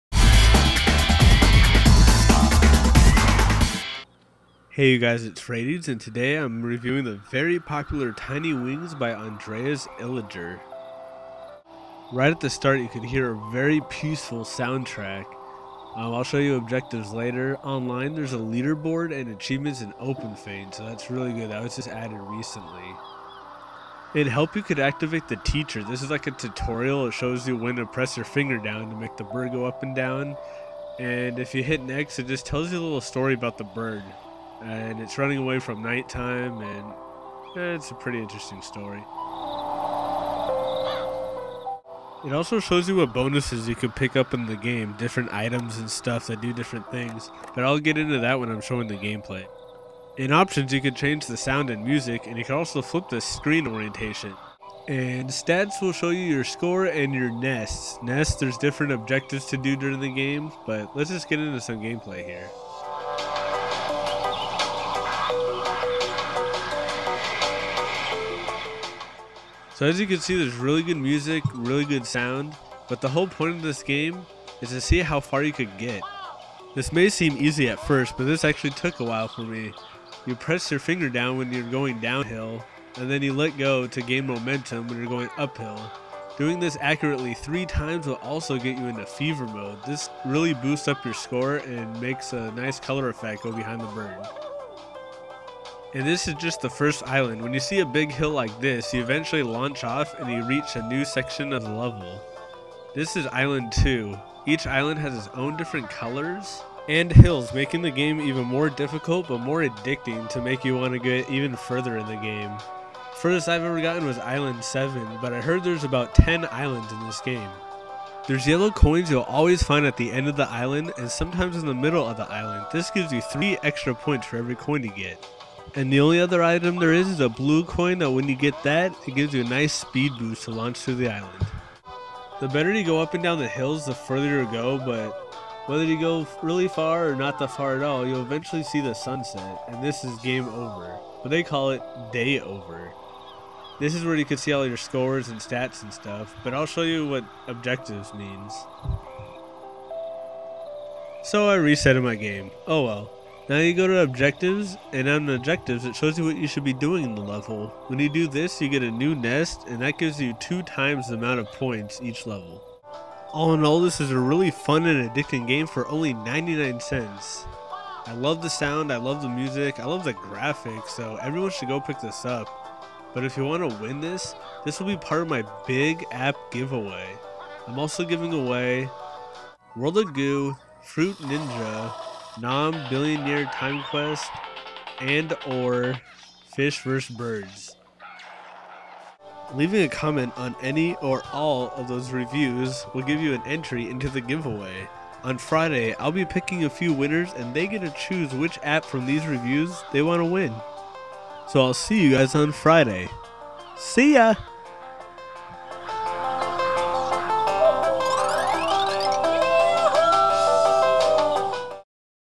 Tiny Wings Review (Giveaway)
Freiteez reviews Tiny Wings by Andreas Illiger

This review is part of the "Big app giveaway" along with World of Goo, NOM: Billion Year Timequest, Fruit Ninja, and Fish Vs. Birds.

Leave a comment to get an additional entry into the giveaway.

Winners chosen on Friday.

Game Center: Freiteez